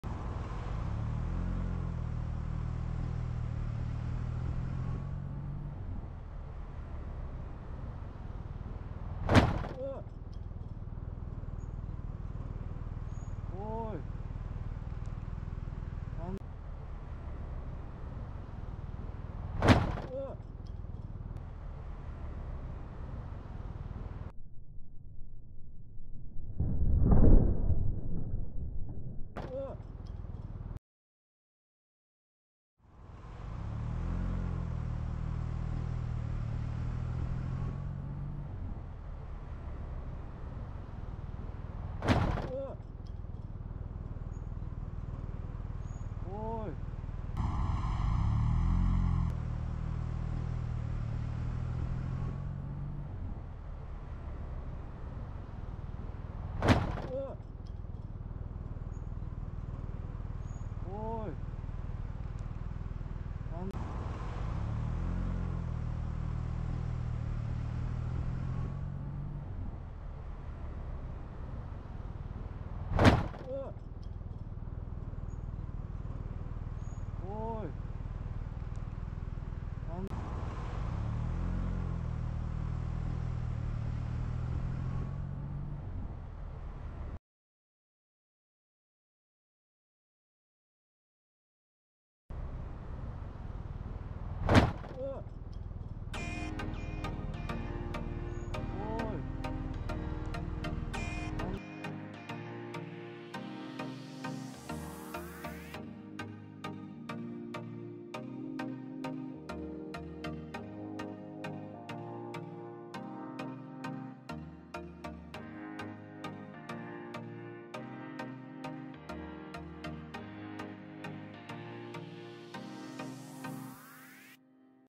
追突で奇跡を起こすバイク乗り 法律に『信号は🟡黄色で安全に止まれ』とある
⚠️こんな事故は初めてです。￼運転免許証を持っている誰もが黄色で安全に止まれる運転ができるから公道を走れ￼ているはずです。どうか黄色で安全に止まる運転を徹底してください。御安全に🫡